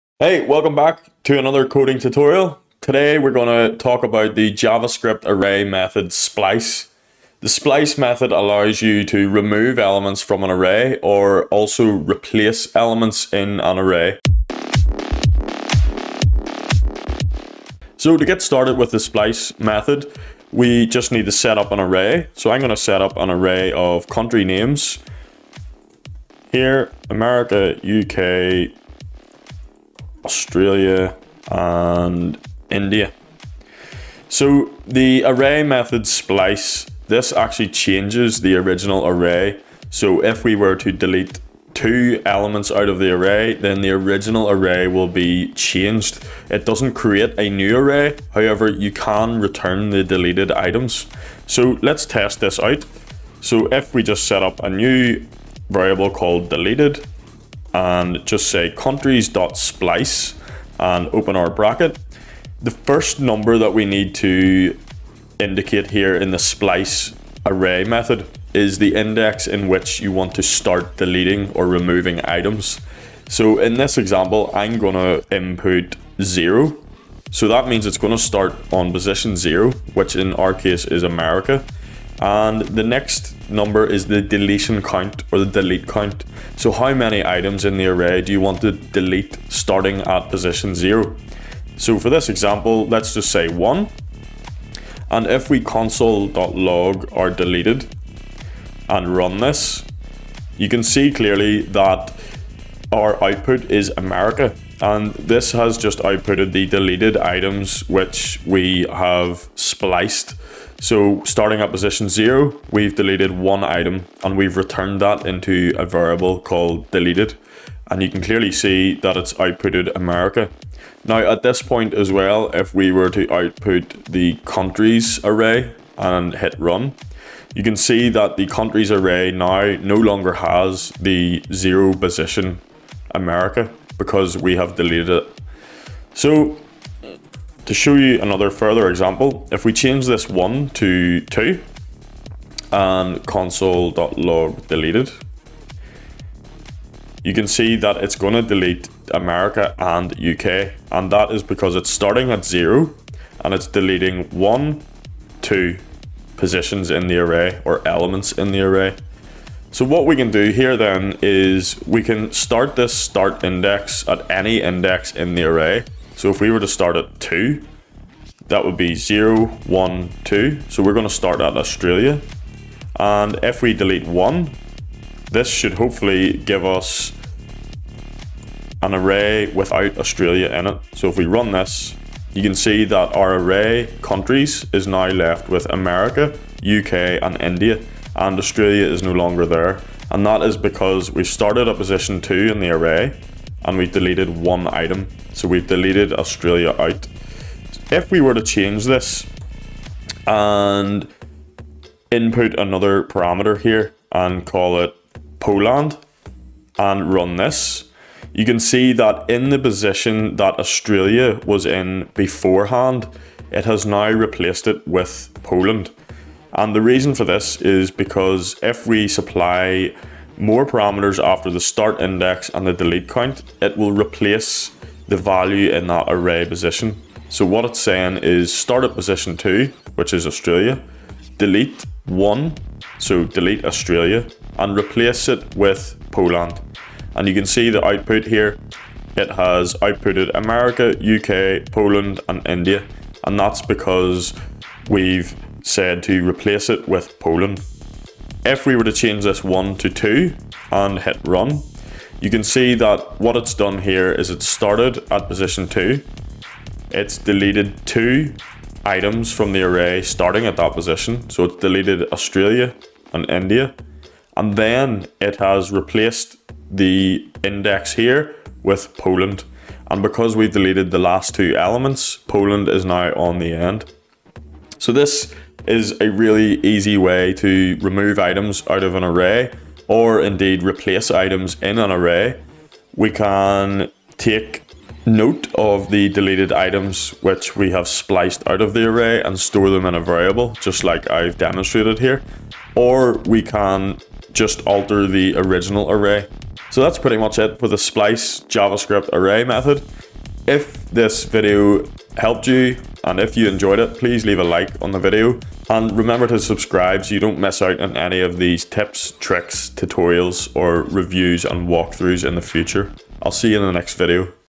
Splice array method in Javascript tutorial
Learn how to use the splice array method in Javascript. This is an array method/function which lets you remove items from an array, or replace elements in an array. This can be the perfect method or function to manipulate a javascript array to delete items.